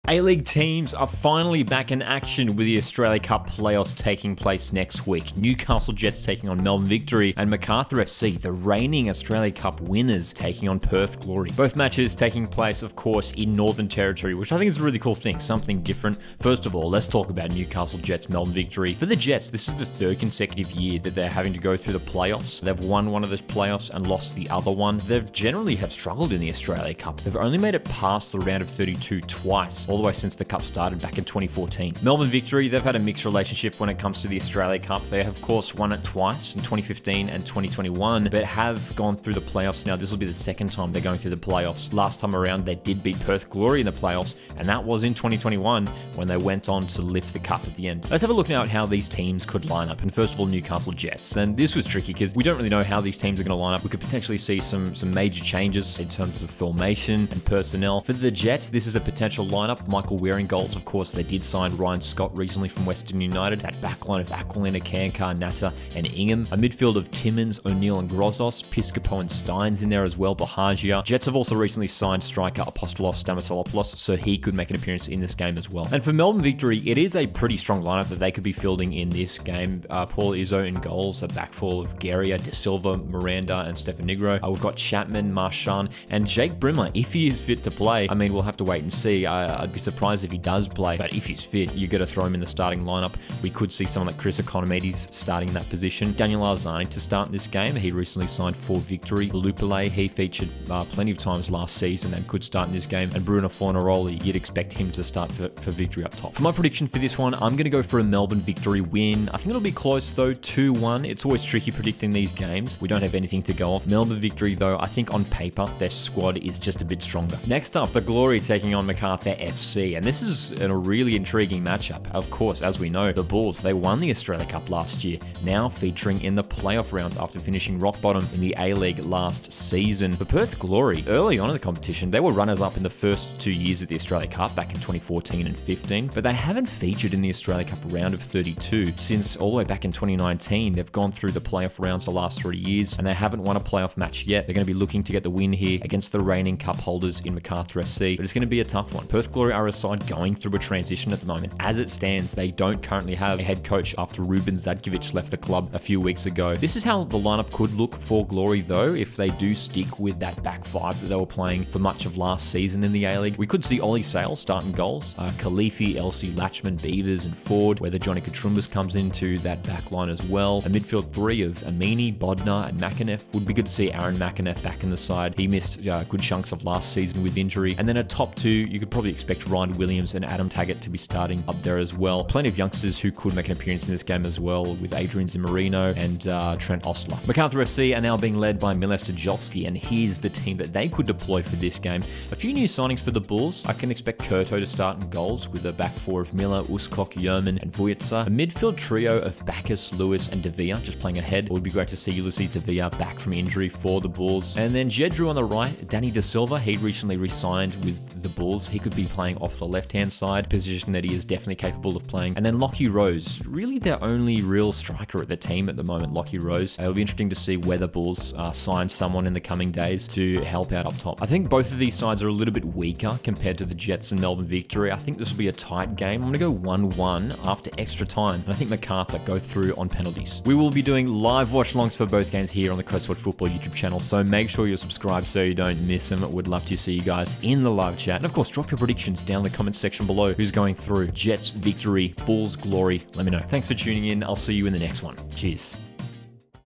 AUSTRALIA CUP | PLAY-OFF PREDICTIONS
The bottom four from the 2022/23 A-League Men season are set to battle for a spot in the Australia Cup Round of 32. Newcastle Jets face 2021 Cup winners Melbourne Victory, while Perth Glory meet current Australia Cup holders Macarthur FC. Here's my predictions for the two matches!

⚽️ JETS v VICTORY | Mon 17 Jul, 7:30pm (AEST)
⚽️ GLORY V MACARTHUR | Tue 18 Jul, 7:30pm (AEST)

0:00 Intro
0:16 Jets v Victory
1:57 Glory v Macarthur
3:50 Outro